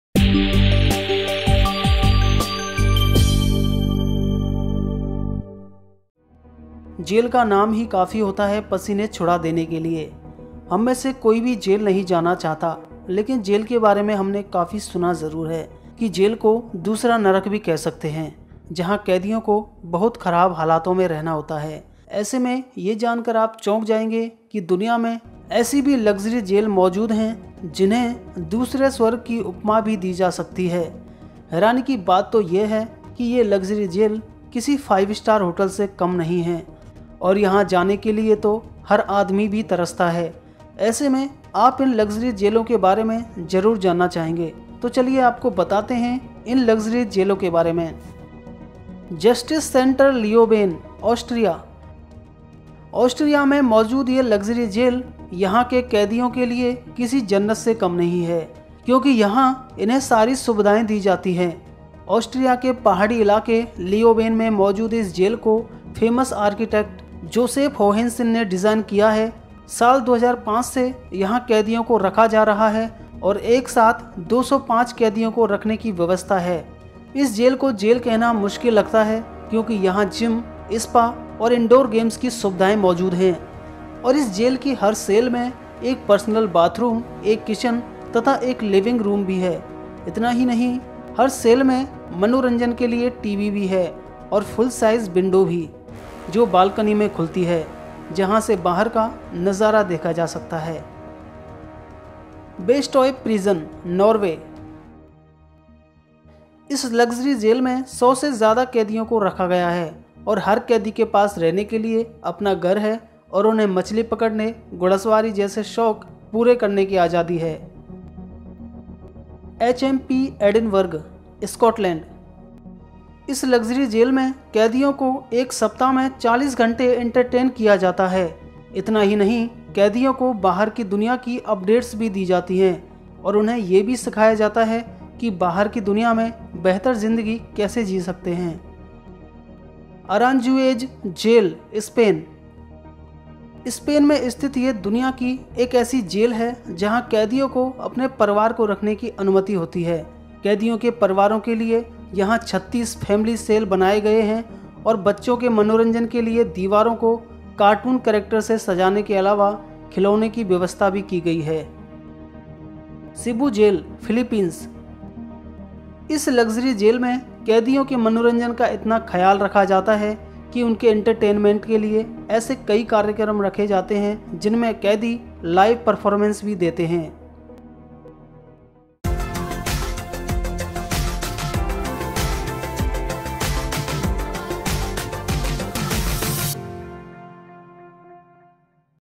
दुनिया की सबसे "LUXURIOUS" जेल । Top 10 Most Luxurious PRISONS In The World. [ HINDI ]
When you think of prison, I bet your thoughts are dominated by images of iron bars, dark hallways, sub-par food and general overall dismay in the air. But what if I told you not all prisons look like that. In fact there are some around the world that, despite being legit prisons, are the exact opposite of what I just described.
Here are some of the world's most comfortable prisons - institutions that have changed how we look at correctional facilities.